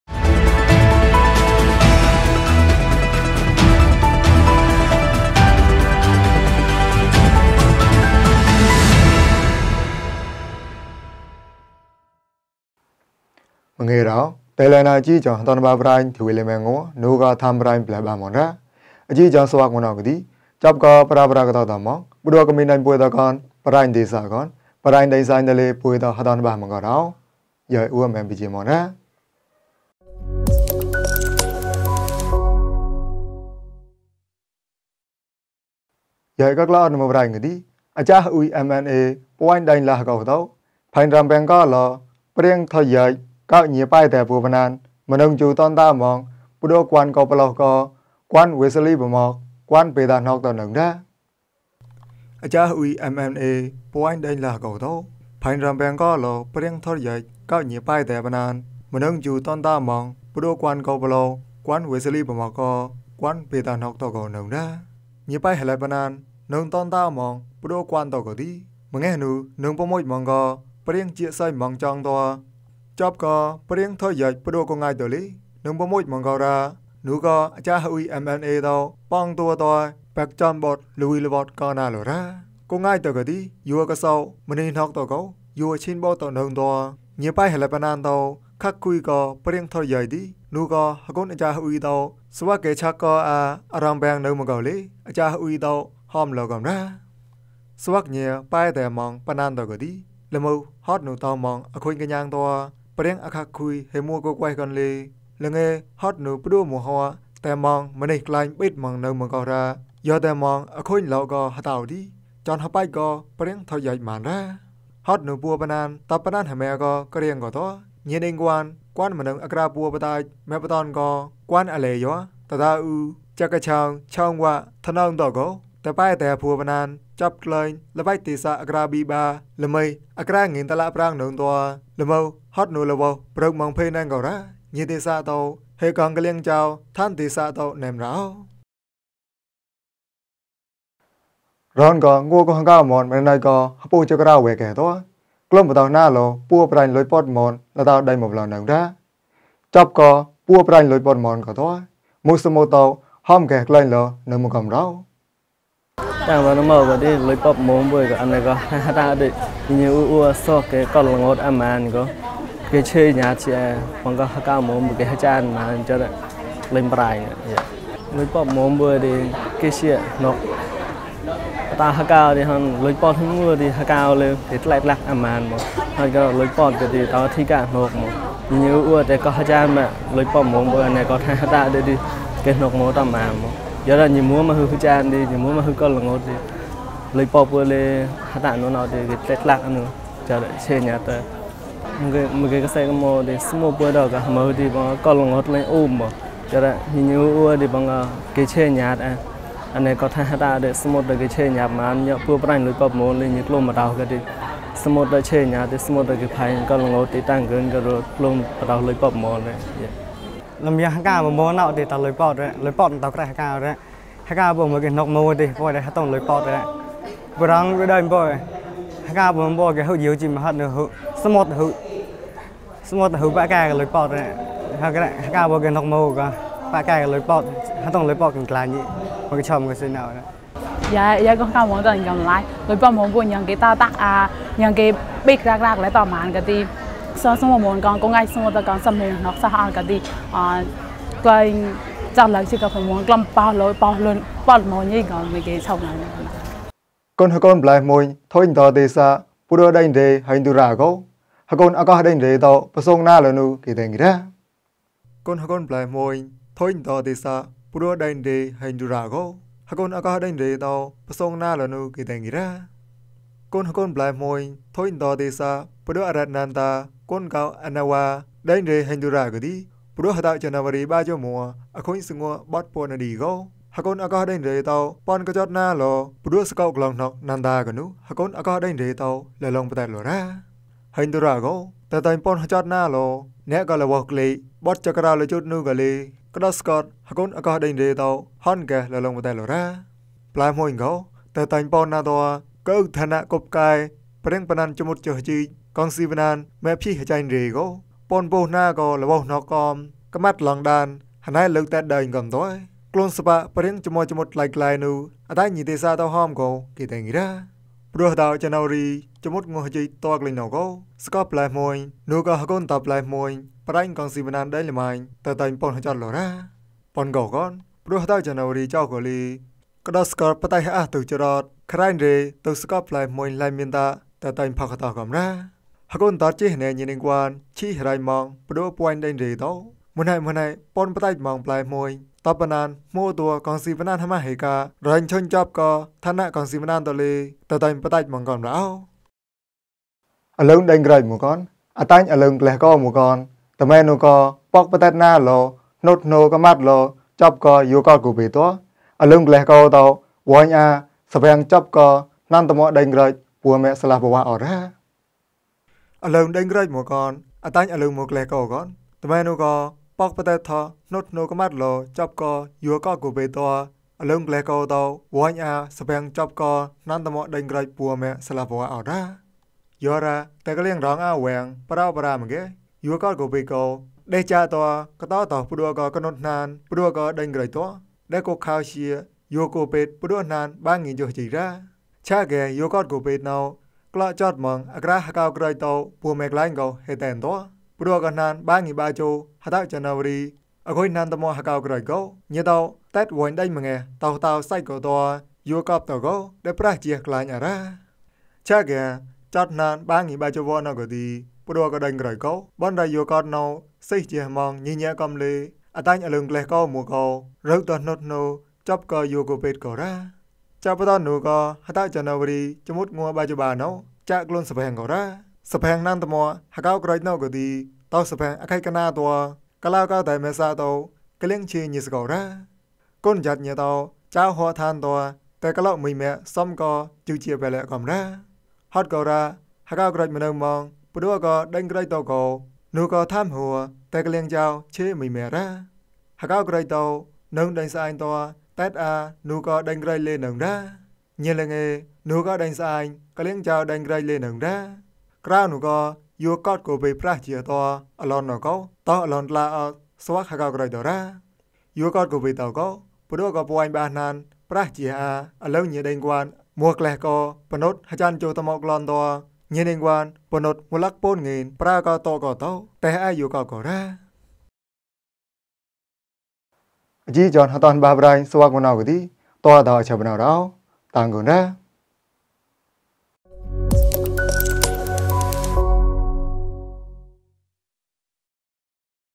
IMNA Daily News -Jan 23.1.2023
ဒုင်တၠုင်ဏာအစဳဇန်ပ္တိုန်ထ္ၜးပရိုင်လၟေင်တ္ၚဲ နူကဵုဌာန်ပရိုင်ဗၠးၜးမန်ရအဴ။
ဂိတုဂျာန်နဝါရဳ၊ ၂၃ ၊ သၞာံ ၂၀၂၃

■အ္စာဂဥုဲမန် MMA ပွိုင်ဍုင်လှာဒကှ် ကဵုအရီုဗင် ပရေင်ထတ်ယုက် ကုညးပဲါဒဴဘဲပၞာန်

■လညာတ်သၟတ် စပ်ကဵုပေဲါပြိုင်လိက်ပတ်မန်

■ပ္ဍဲဍုင်ရေဝ် ဗၠာဲသၟိင်ထိင်ဒဝ်ဒေသ ဟုင်တူရ ဒးဒုင်ပန်ဂစိုတ်

■ရုပ်ထောံသၞောတ်ဝ် စပ်ကဵုယဲကိုဝ်ဝေဒ်တုဲ အလုံမွဲဂၠးကဝ် ဝေင်သဘင်သၞာံတၟိကြုက်